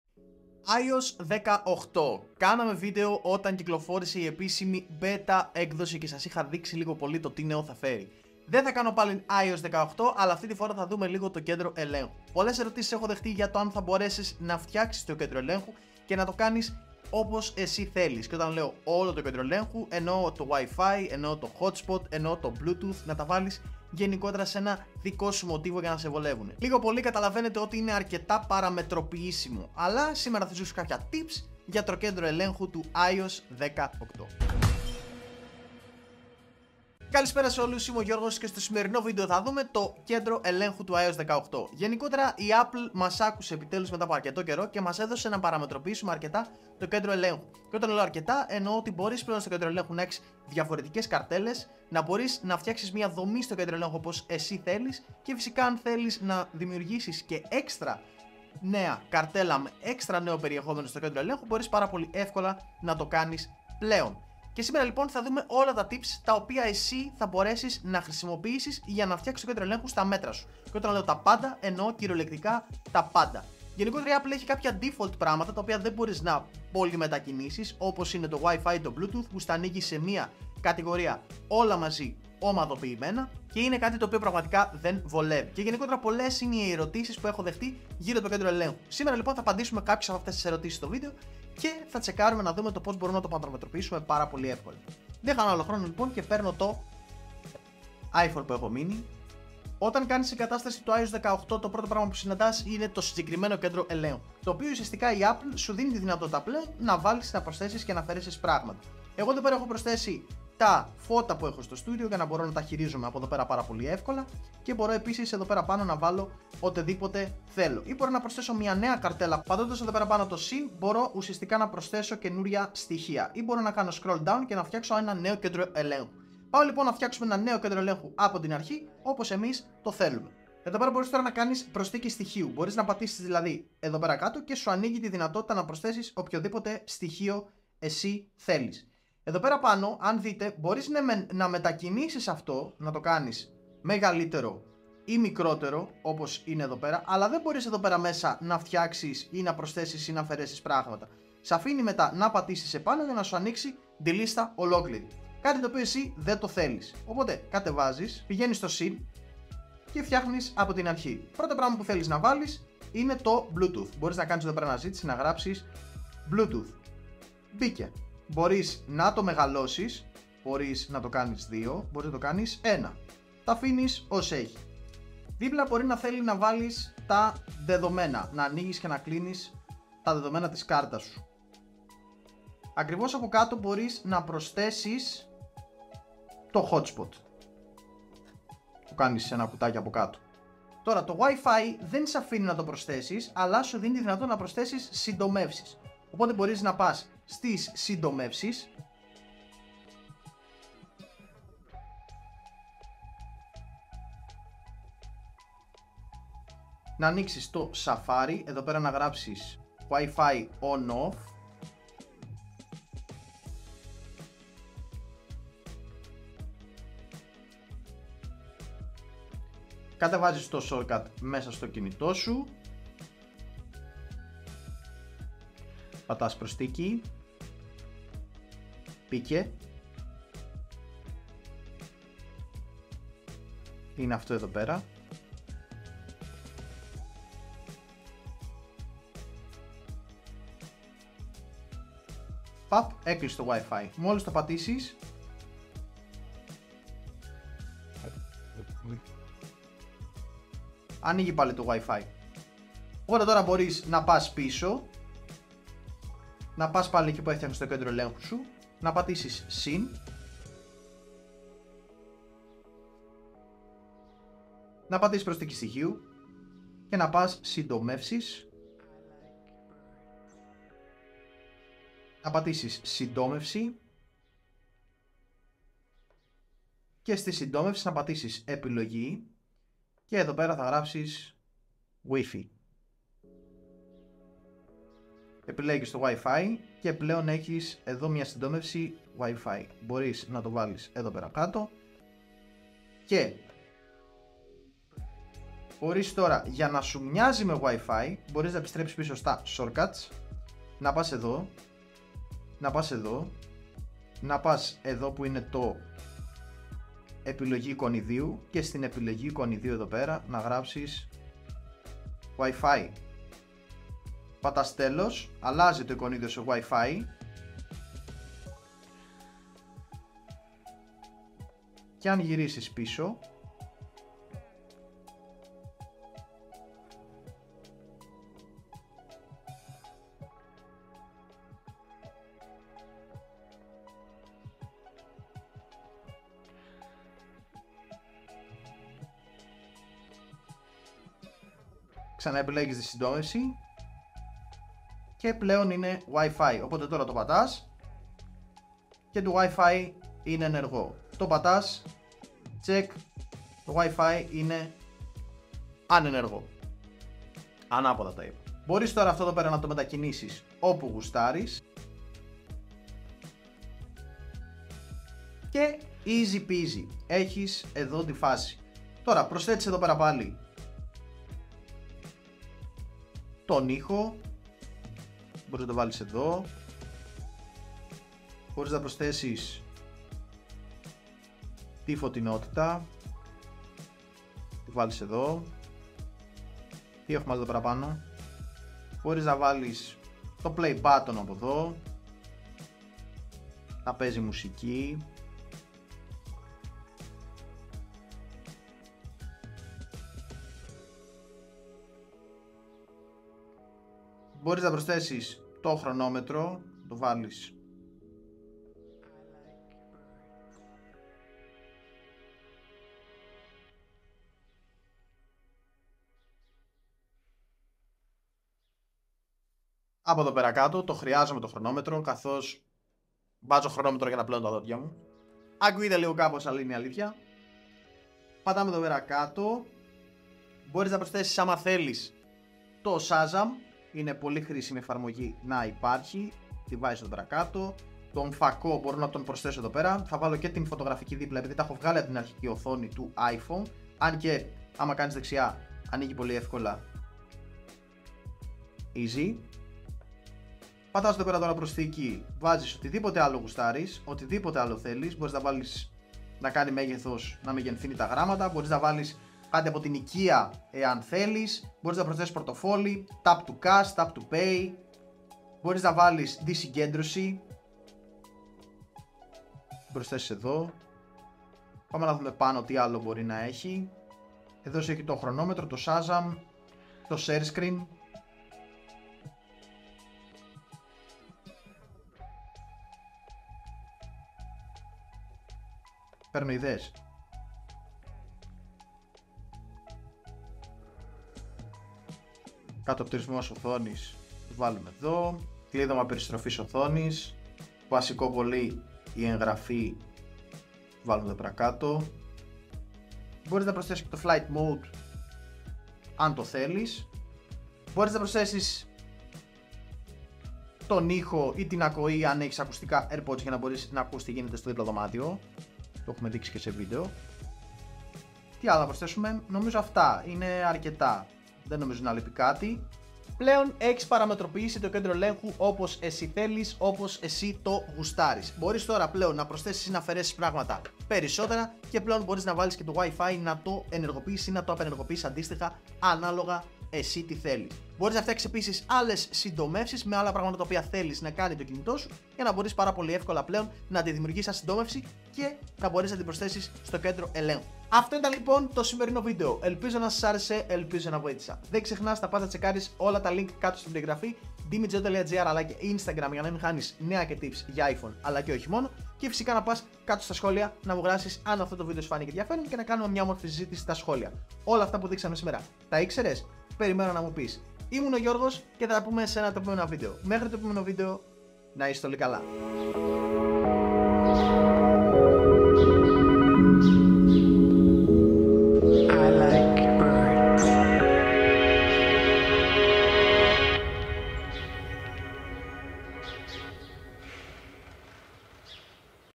iOS 18 & Cotrol Center
Εξοπλισμός :
Κάμερα :
Sony A6300

Lens:
Sony 16-70 ZEISS 40f

00:00 Introduction
00:35 Intro
01:37 τι θα δούμε
03:03 βασικά πράγματα διαμόρφωσης
06:11 Πως να βάλεις το WiFi
08:44 Κάπως έτσι μοιάζει το νέο Κέντρο ελέγχου
09:17 Κλείσιμο